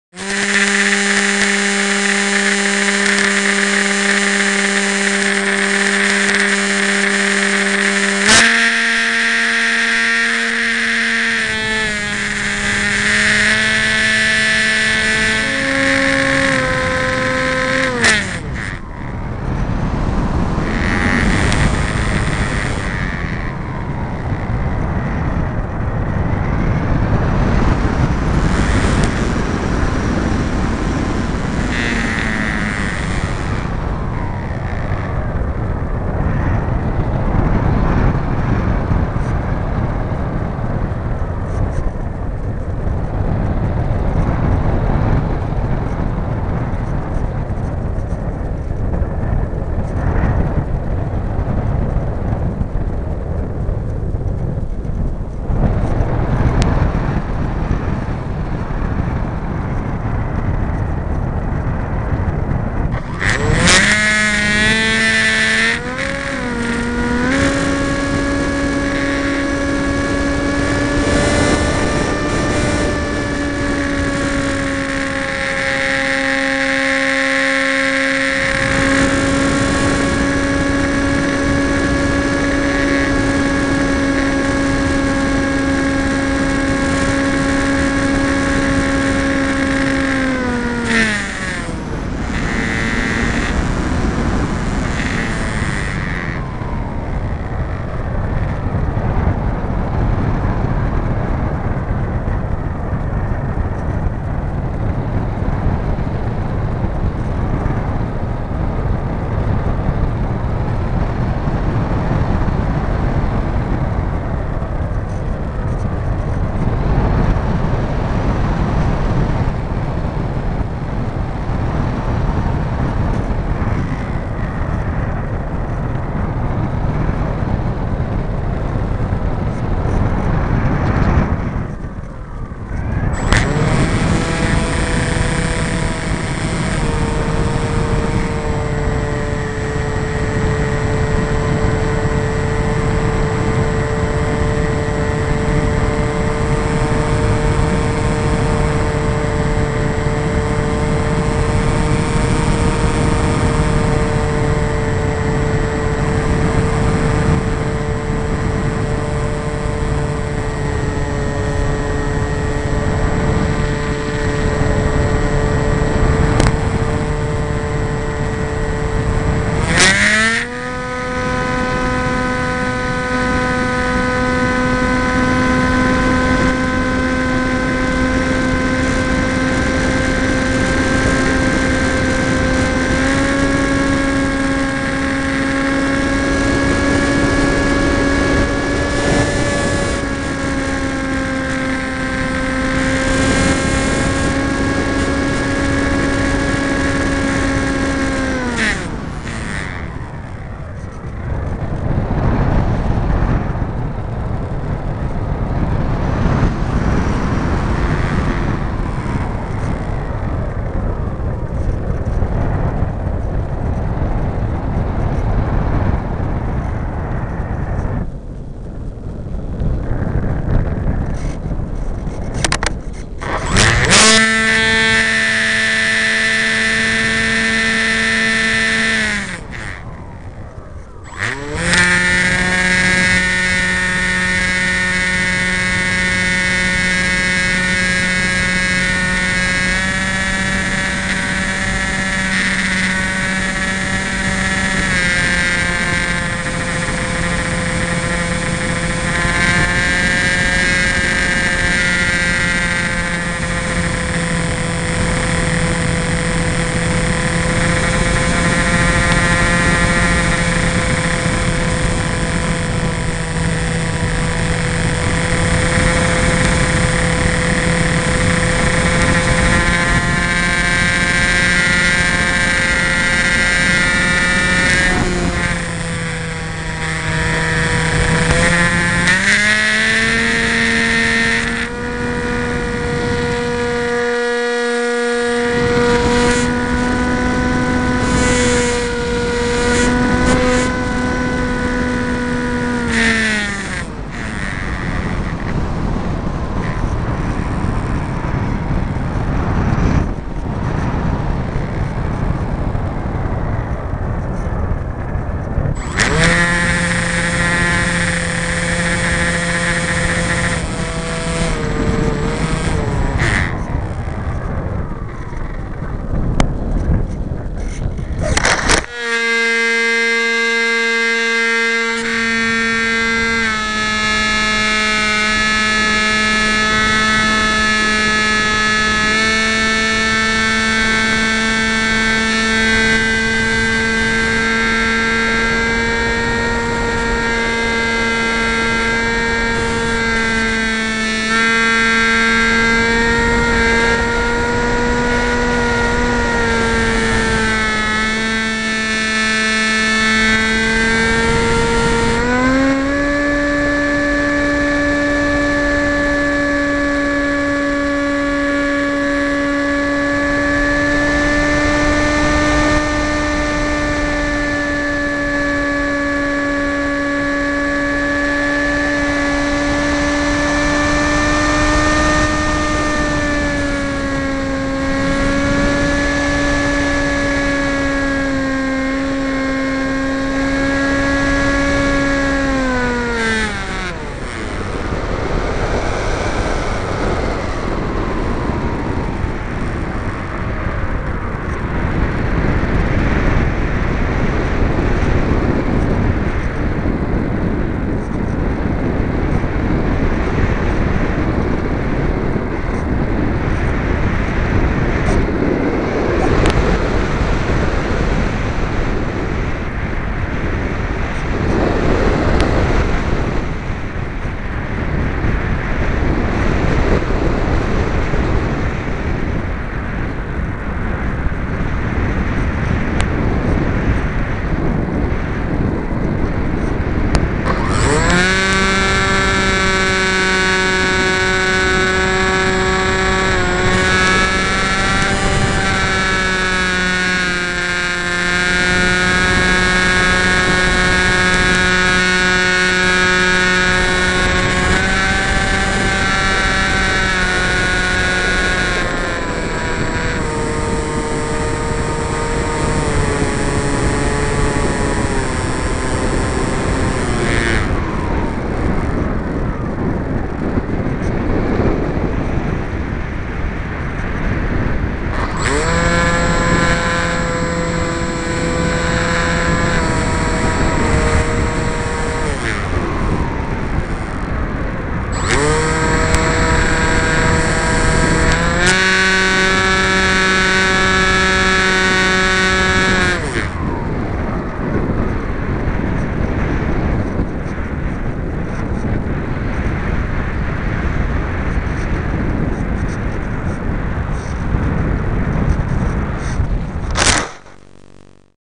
AXN Learning 2 Fly - Day 3
Just some footage of the thirth day that I was self-learning to fly my AXN.
Nice day with calm winds, all went well on this day.

The cam on the wing is a 808 #6 so poor quality. New 808 #16 720P is ordered.